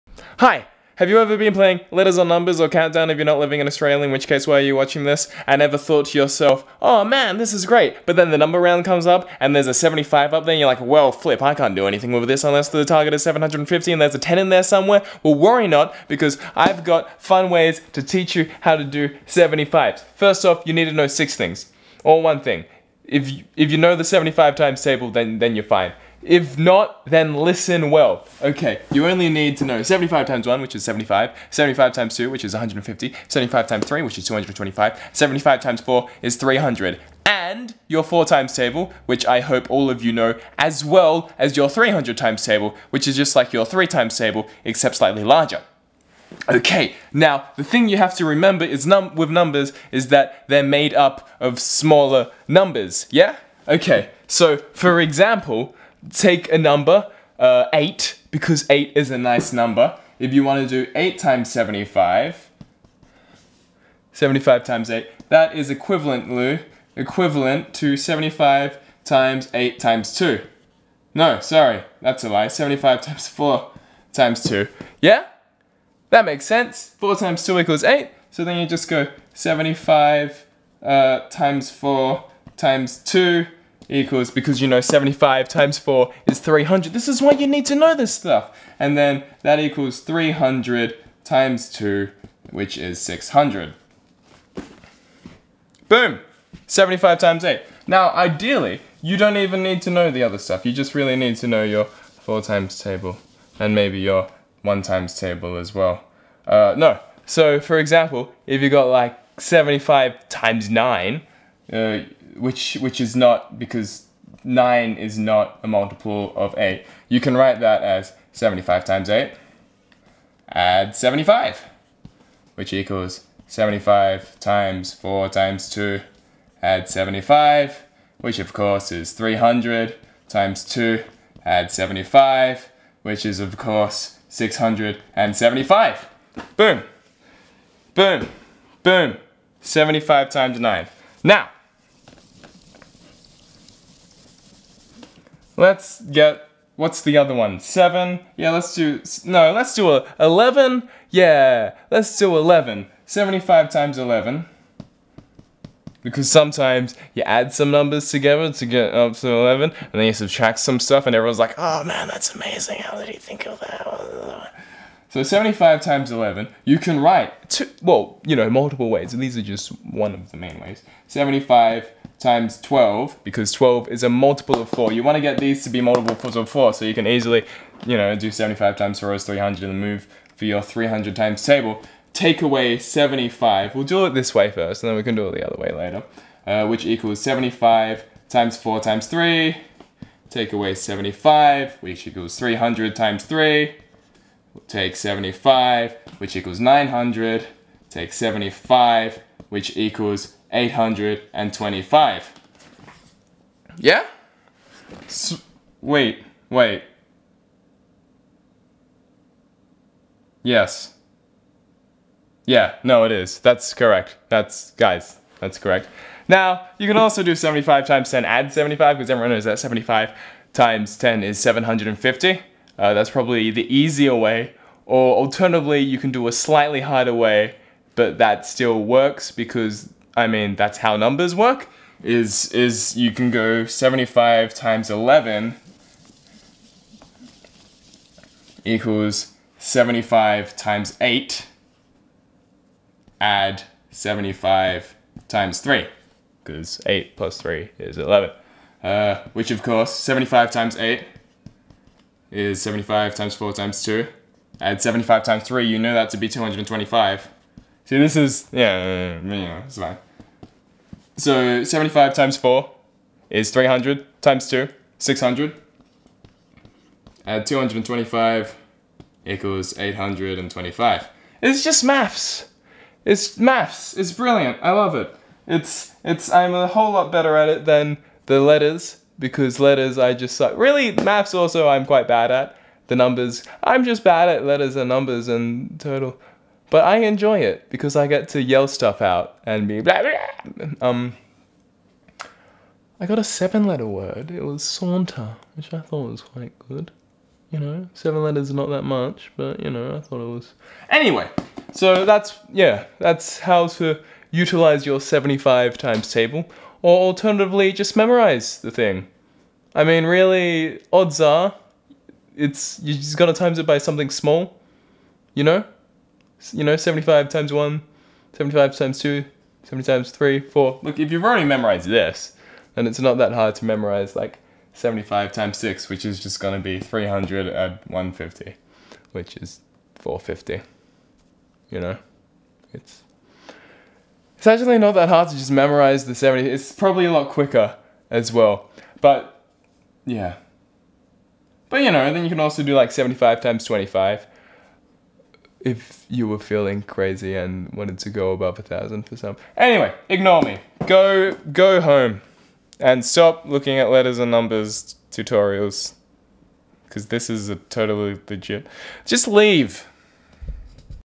How to multiply by 75
Have you ever been watching Letters and Numbers or Countdown and been like, "Man, I wish I could use that 75".
Well now you can! In a lengthy and really unnecessary way. Really you are better off just memorising the times table.